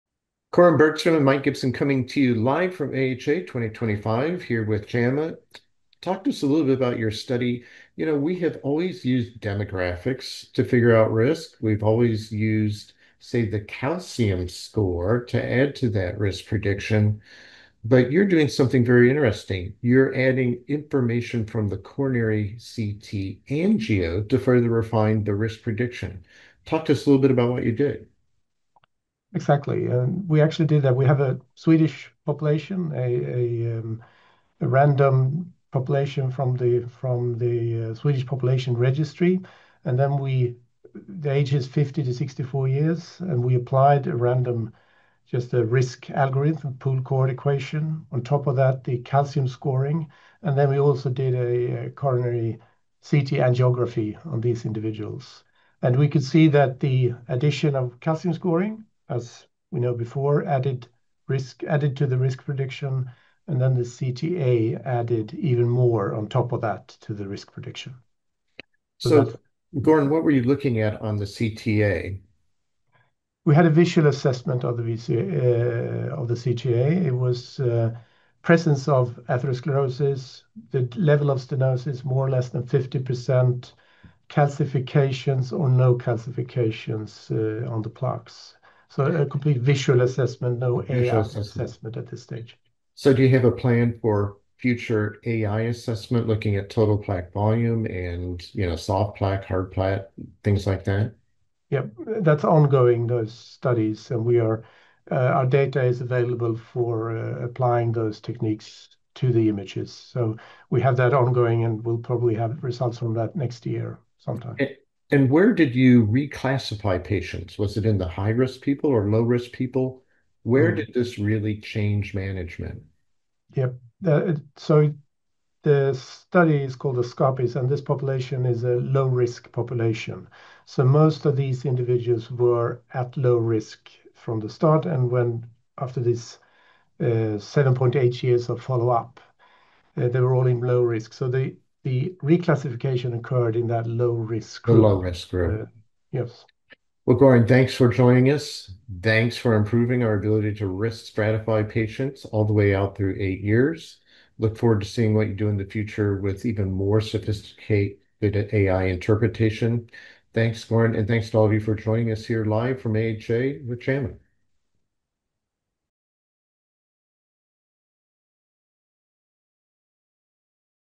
Coronary Computed Tomography Angiography in Prediction of First Coronary Events
Adding coronary CT angiography to traditional risk scores modestly improved prediction of first coronary events in a large population-based study.